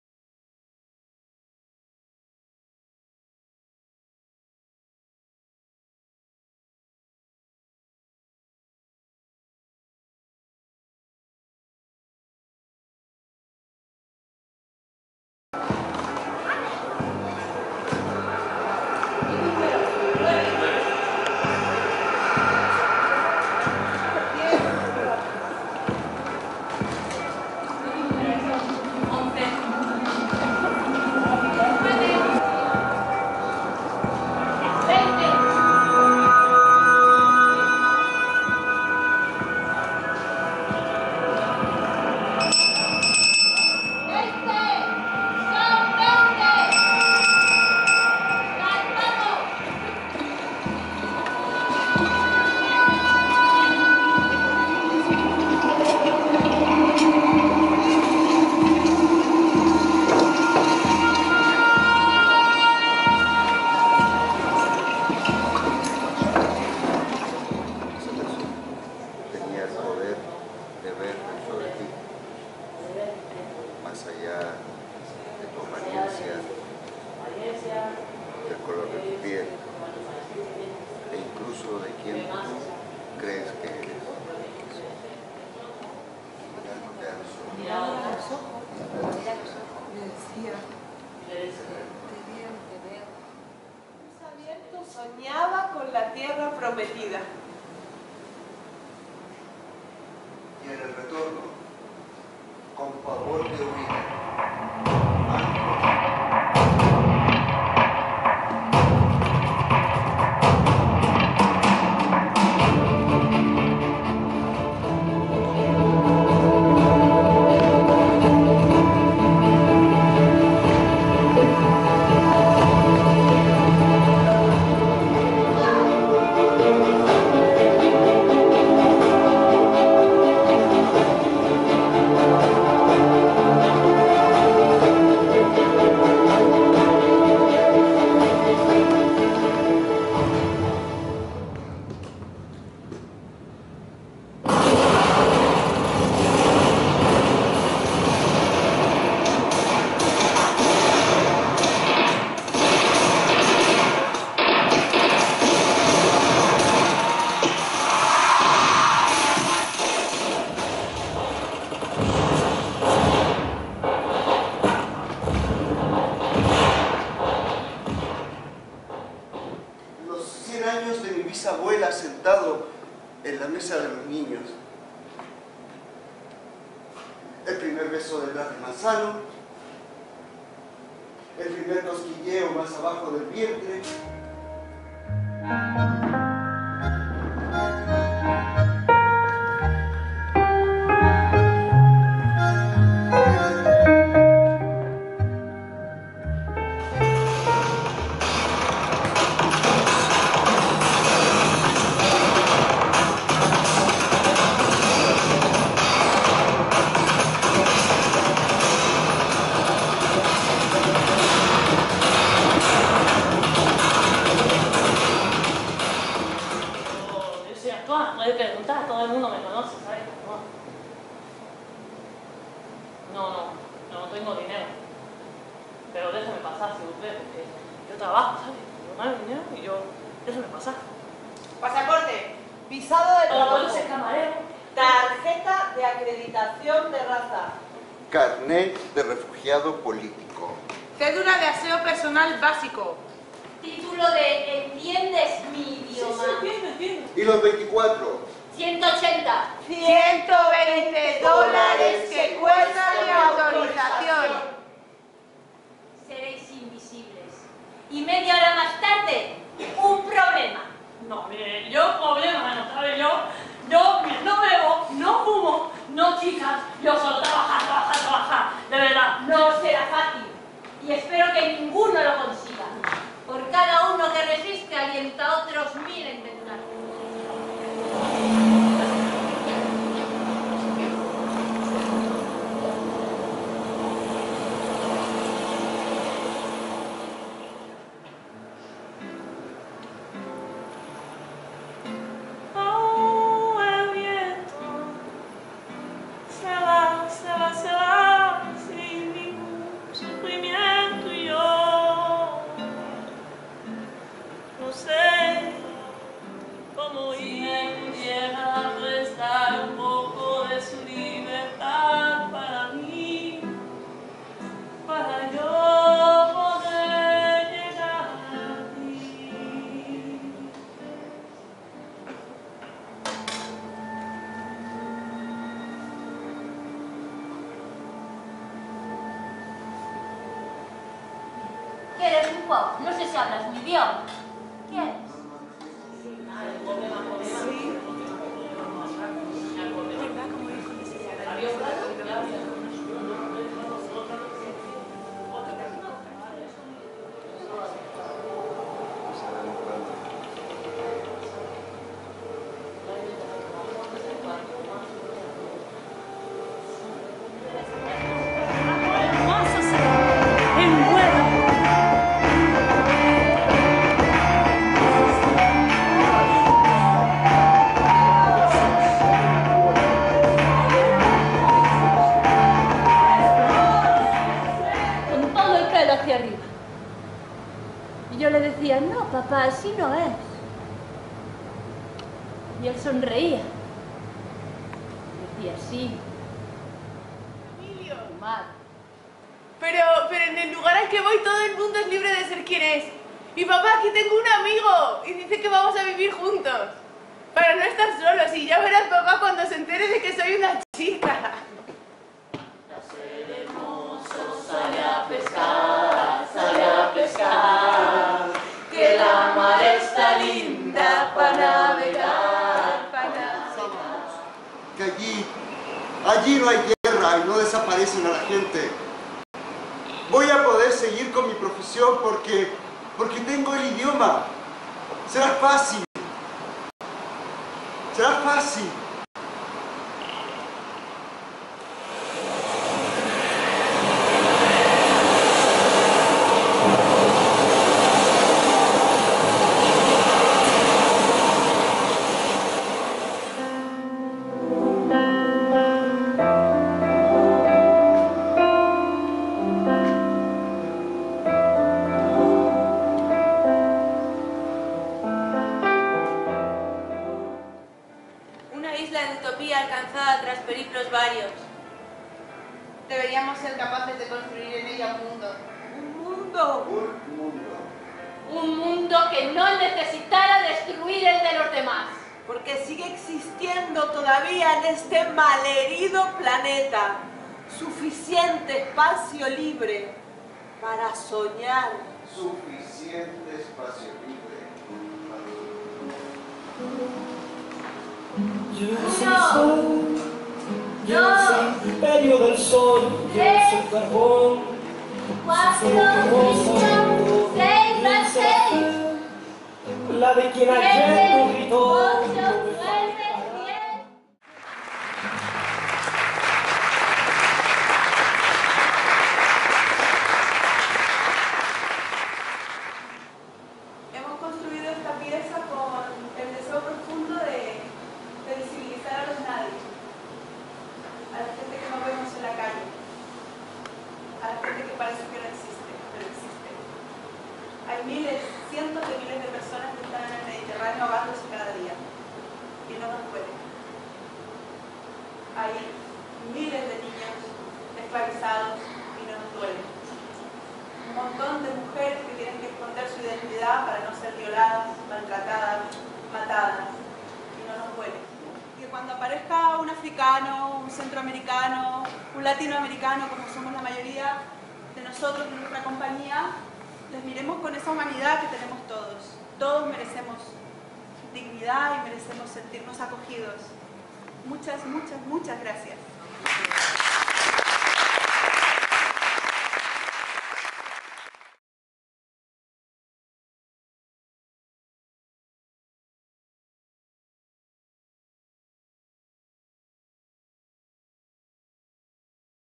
Pasos Perdidos (Teatro en el Aire)
"Hemos construído esta pieza con lel deseo profundo de visibilizar a los nadie..."

"Pasos perdidos", de la compañía Teatro en el Aire (Madrid)
Dirección artística y dramaturgia: Lidia Rodríguez Correa y Teatro en el aire.
Intérpretes: Carlos Javier Sarmiento, Rodrigo Villagrán, Guadalupe Marcote, Laura de Casas, Barbara Gómez, Rocío Herrera y Lidia Rodríguez.
(2017/12/17 - Pabellón 6, Bilbao)